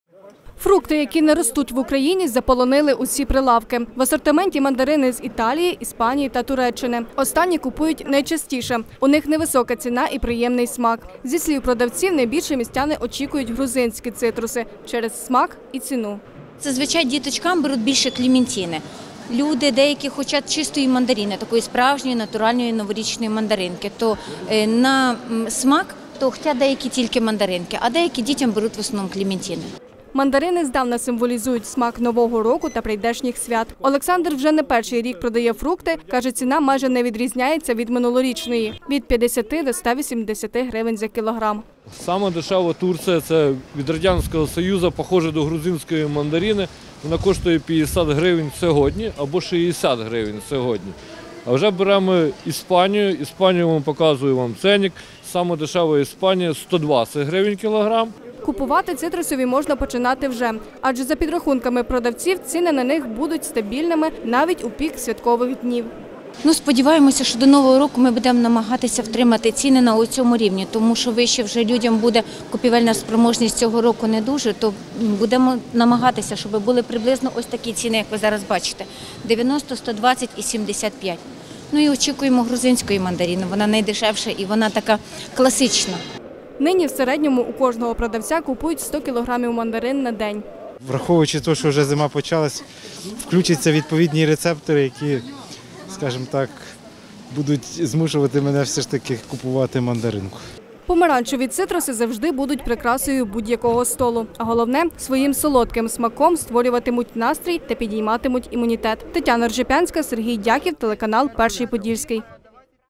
Яка ціна на мандарини цього року?
Сезон мандаринів стартував. У Хмельницькому кожен овочевий кіоск та ринок заповненні помаранчевими вітамінами. Скільки коштує новорічний фрукт і чи очікувати підвищення цін до новорічно-різдвяних свят?